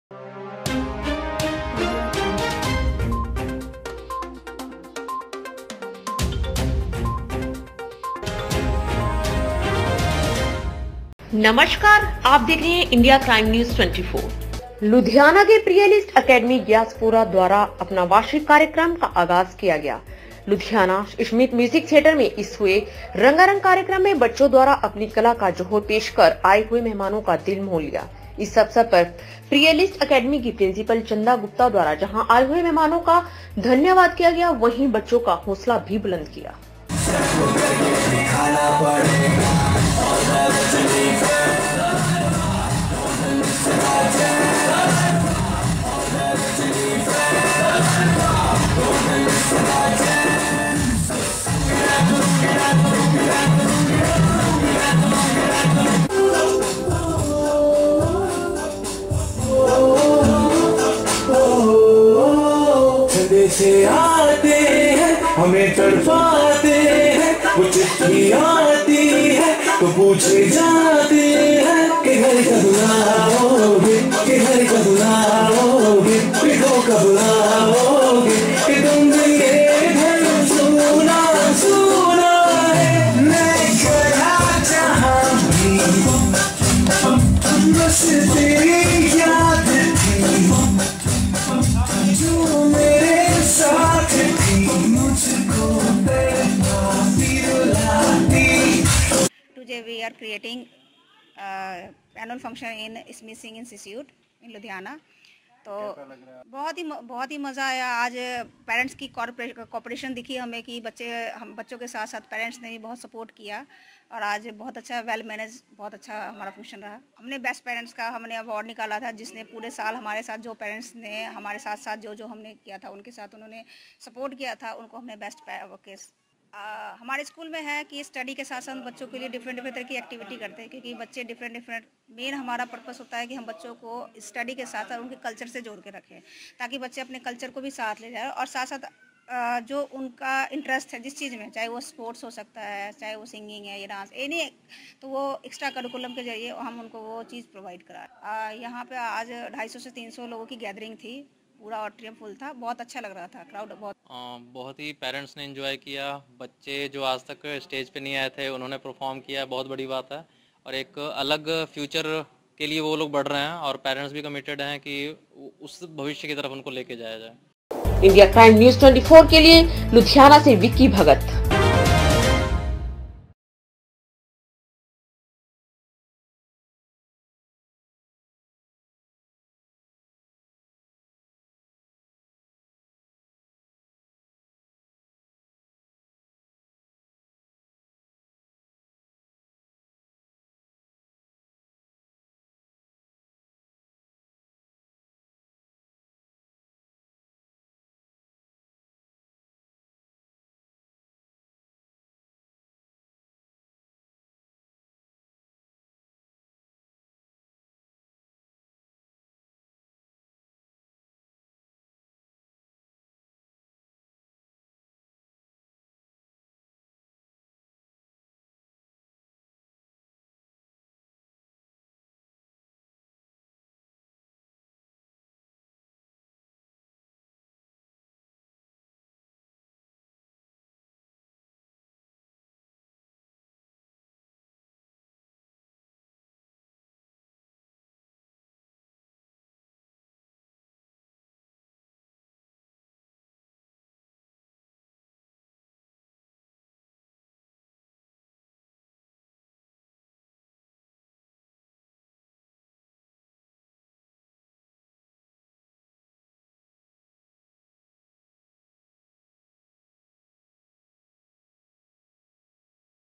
लुधियाना के प्रियलिस्ट एकेडमी गैसपुरा द्वारा वार्षिक कार्यक्रम का आगाज India Crime News24 Reporter V
लुधियाना के प्रियलिस्ट एकेडमी गैसपुरा द्वारा वार्षिक कार्यक्रम का आगाज-India Crime News24 Reporter Vicky Bhagat